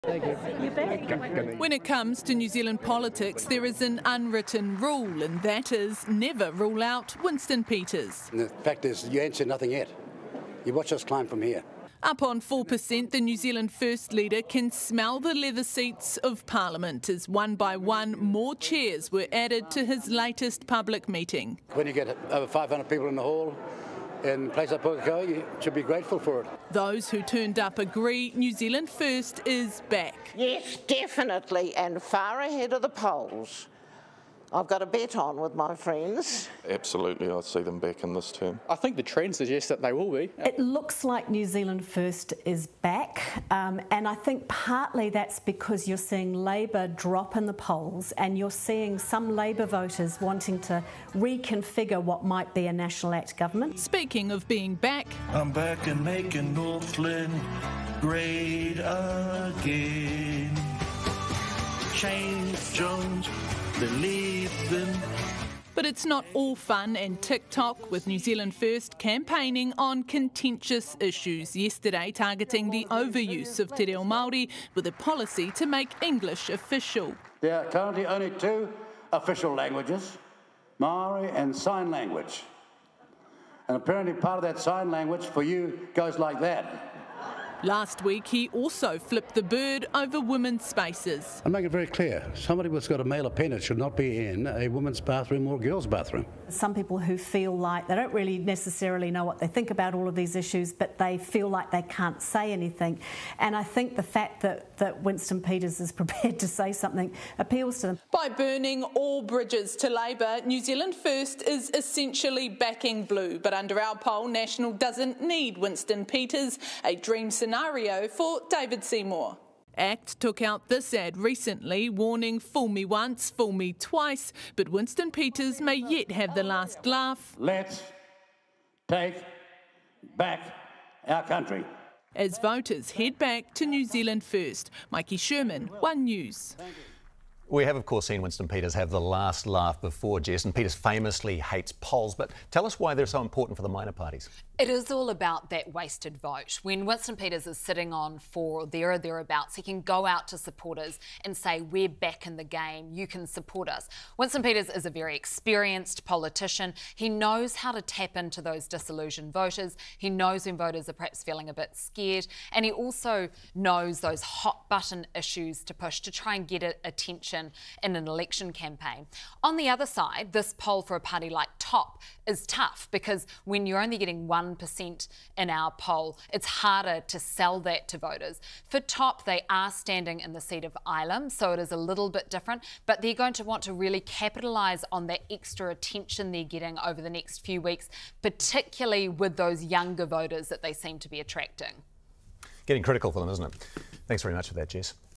Poll: Winston Peters closing in on a return to Parliament
New Zealand First have climbed to 4% in the polls, just 1% shy of the threshold needed to get a seat in Parliament.